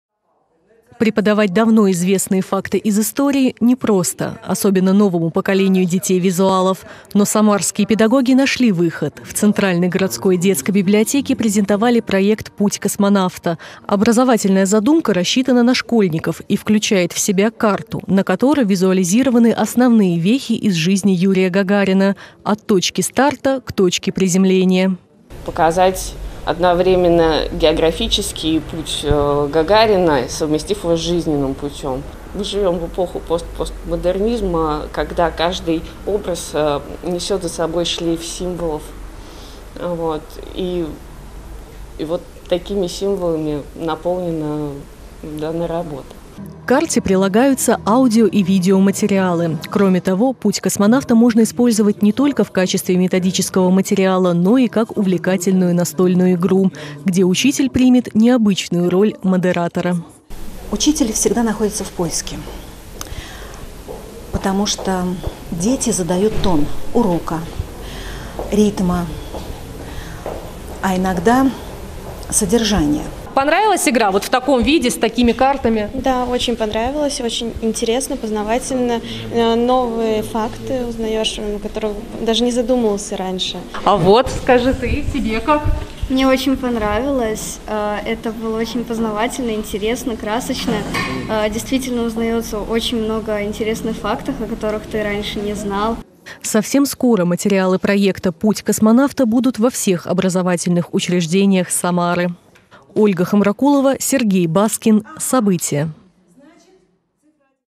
В Самаре разработали креативный образовательный проект "Путь космонавта"
Чтобы урок о первом полете человека в космос был нестандартным, в Самаре разработали креативный образовательный проект "Путь космонавта". Подборка аудио- и видеоматериалов с красочной авторской картой могут быть не только методическим материалом, но и настольной игрой.